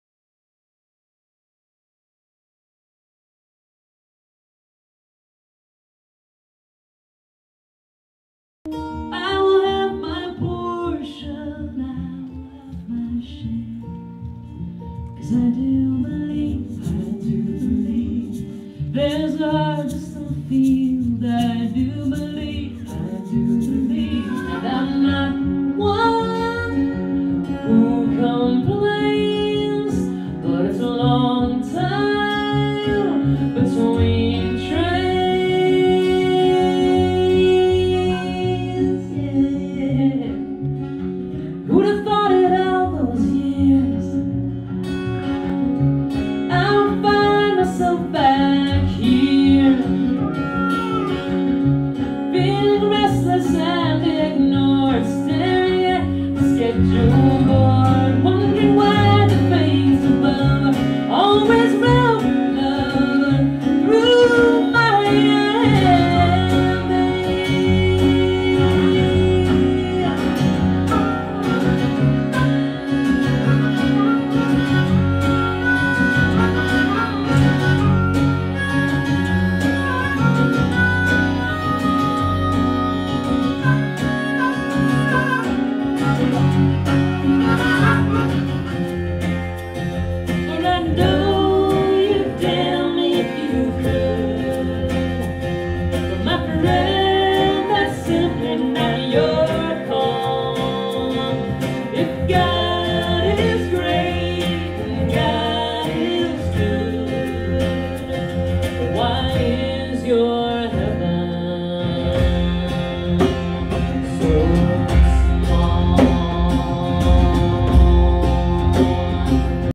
Susan Werner and Trina Hamlin at Kerrville 5/28/07
The amazing Susan Werner and Trina Hamlin onstage for the first night of The Kerrville Folk Festival.